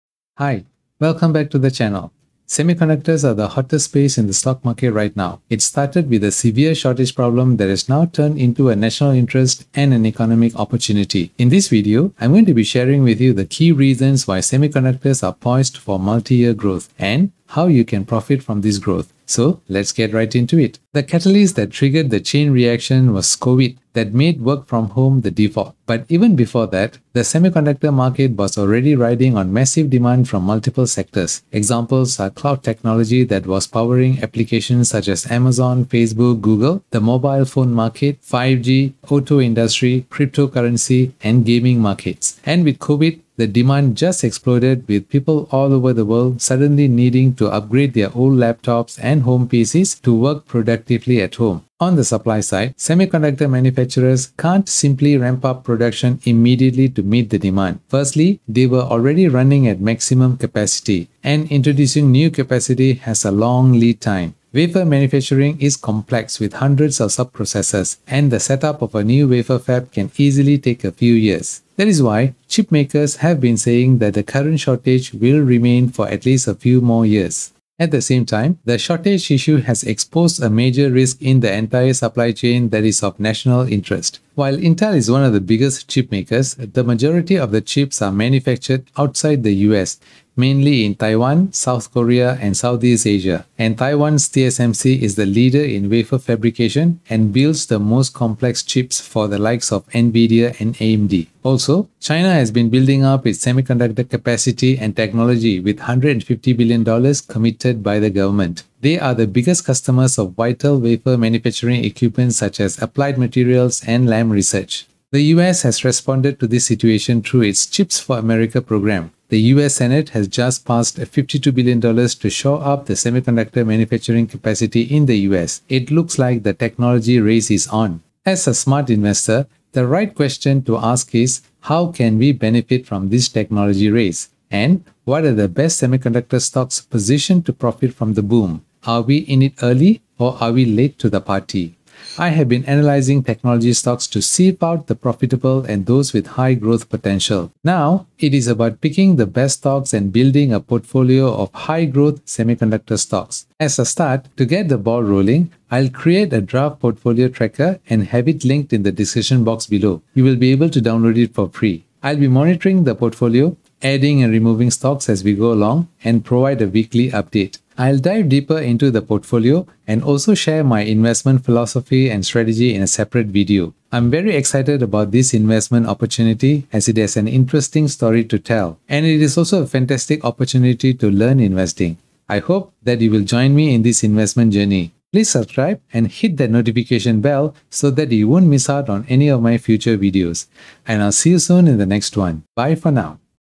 Semiconductor Stocks Portfolio | Best Semiconductor Stocks to Invest in 2021
The semiconductor market is poised for multi-year growth driven by over demand, under capacity, and strategic national interests.

In this Introduction video, I discuss the undercurrents that are lifting the semiconductor market.

In the next video in this series, I'll dive deeper into the High-Growth Semiconductor Stocks Portfolio and share my Investment Strategy to profit from this boom.

👍 And here are my Most Popular Recent Videos,

and learn from other like-minded investors.